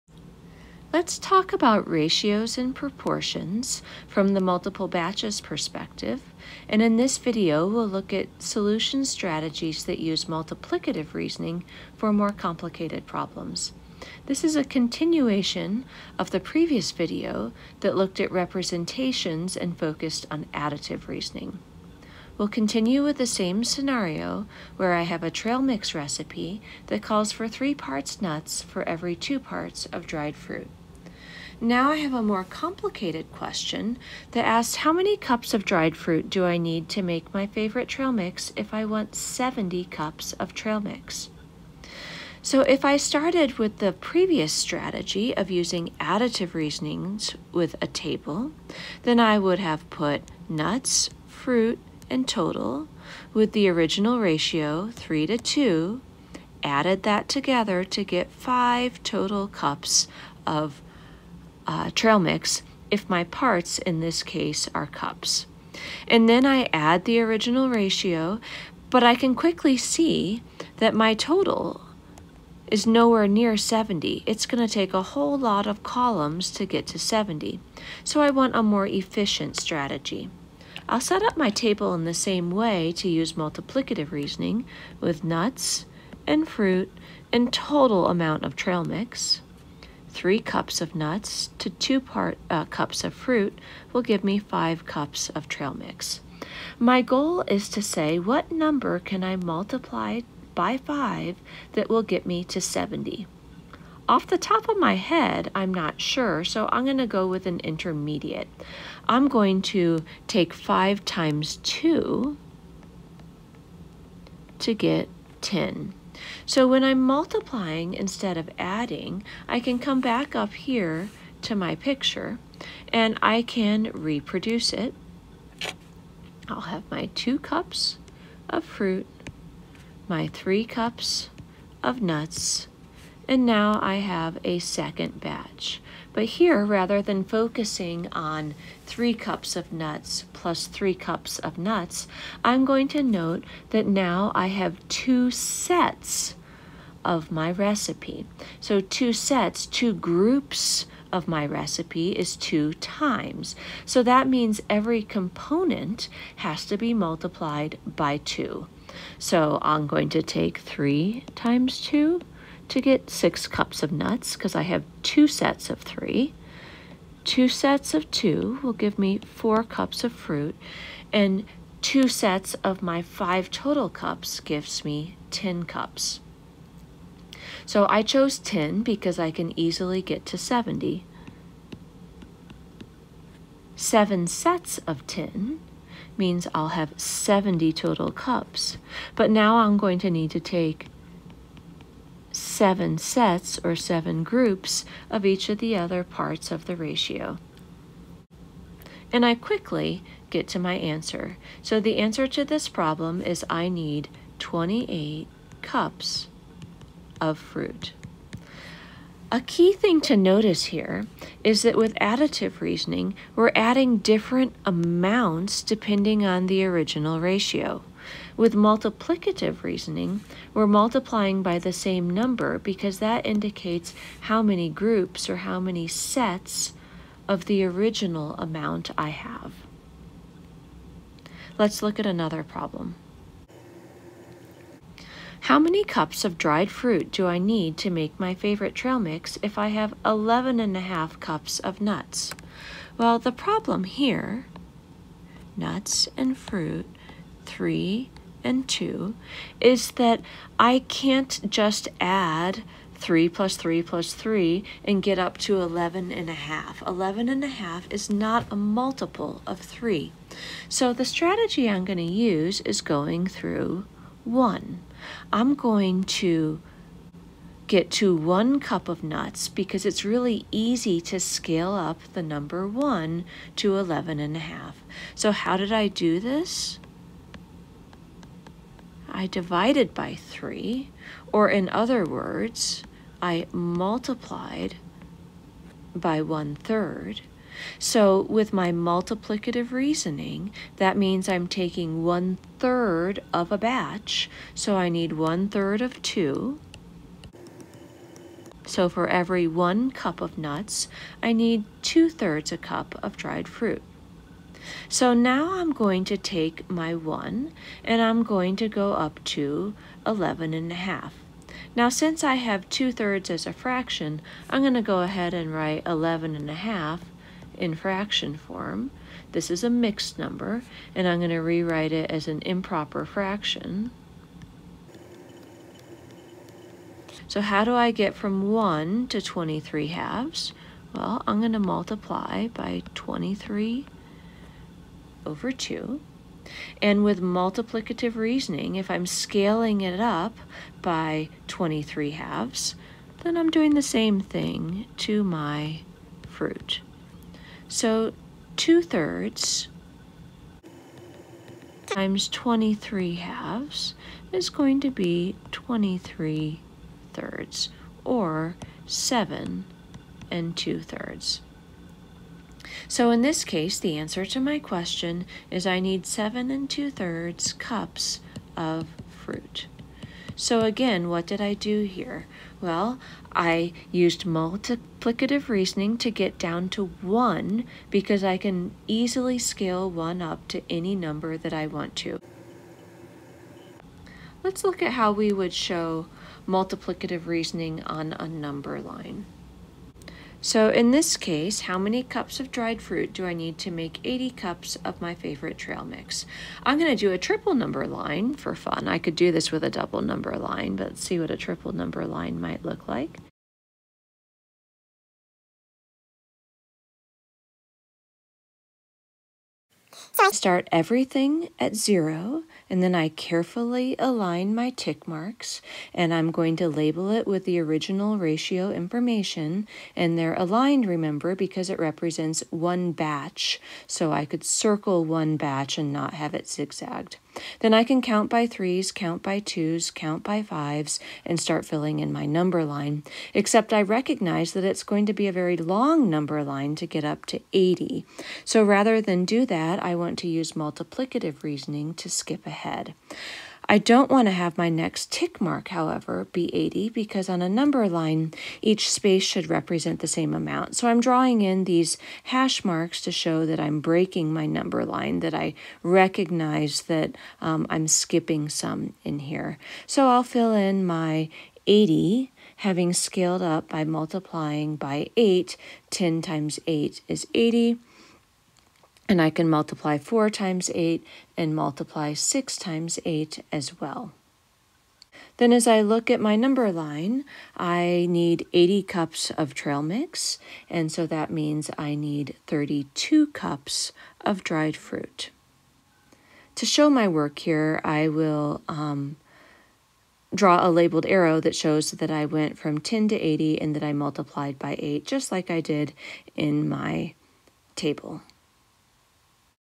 Ratios and Proportions: Multiple Batches - solution strategies and multiplicative reasoning
This is a continuation of the previous video.  We look at more difficult R&P problems and use multiplicative reasoning with table and number lines.  The important strategy of going through 1 is discussed.